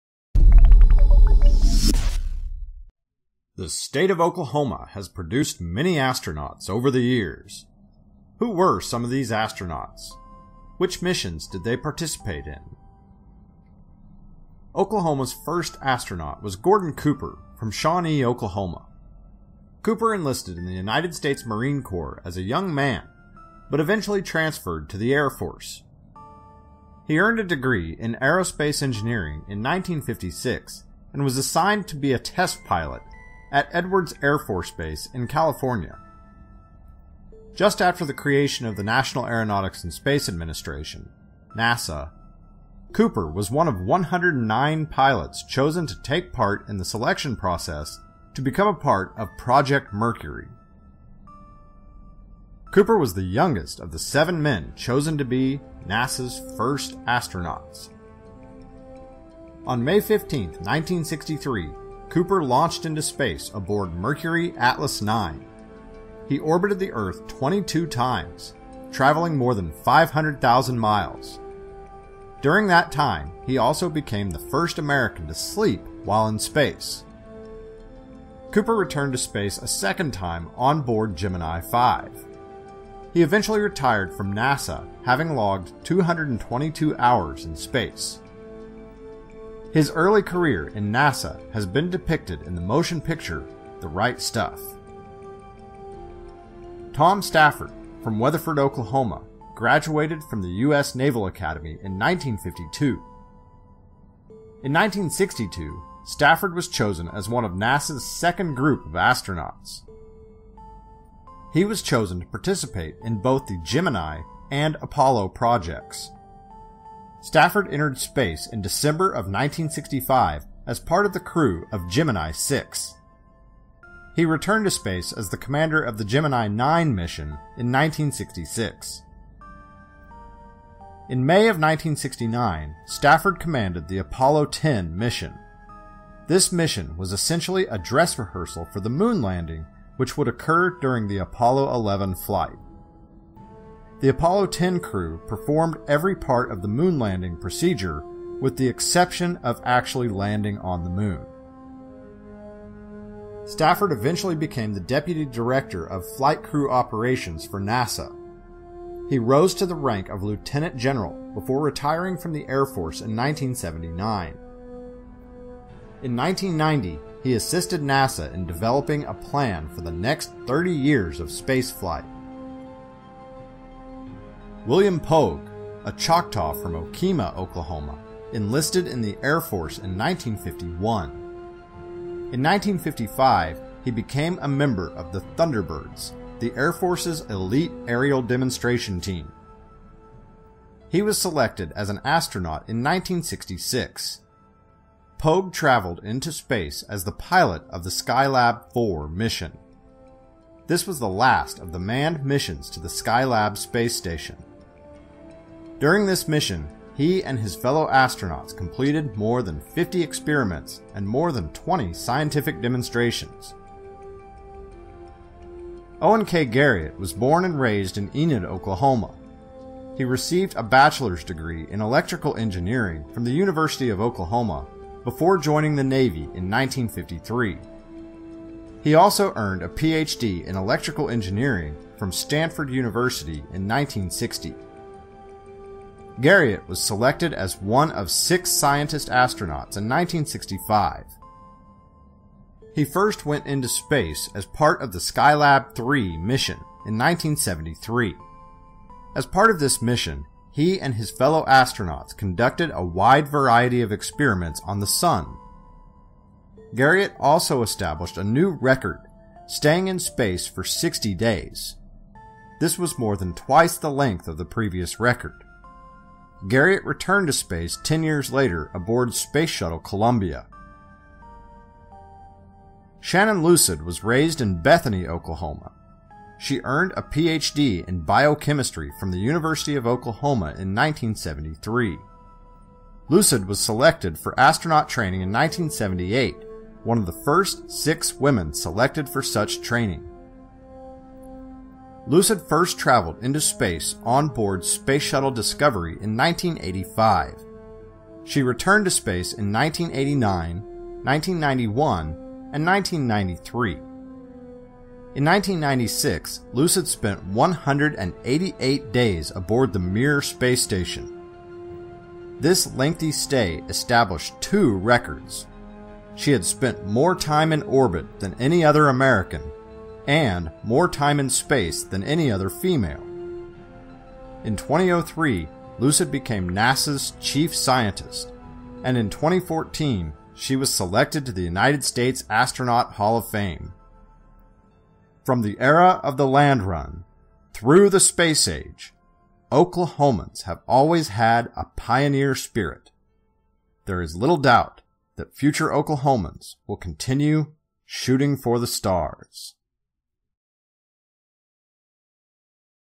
Oklahoma History: Oklahoma Astronauts
This video gives a brief description of Oklahoma's five most famous astronauts.  For teaching materials covering these products, visit our